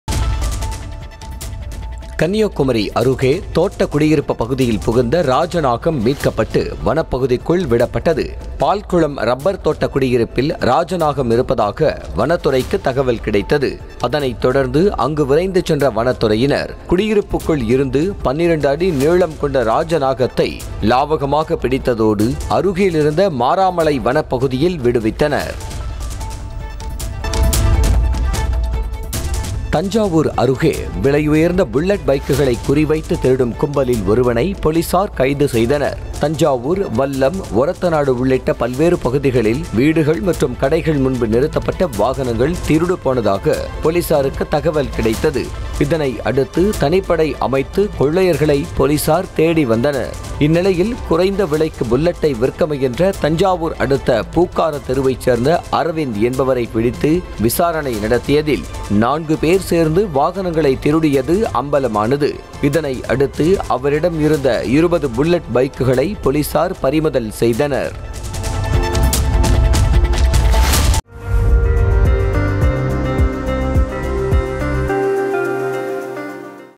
தோட்ட குடியிருப்பு பகுதியில் புகுந்த ராட்சத ராஜநாகம்.. லாவகமாக பிடித்த வனத்துறையினர் | Kanyakumari
தோட்ட குடியிருப்பு பகுதியில் புகுந்த ராட்சத ராஜநாகம்... லாவகமாக பிடித்த வனத்துறையினர் | Kanyakumari | Snake Resuce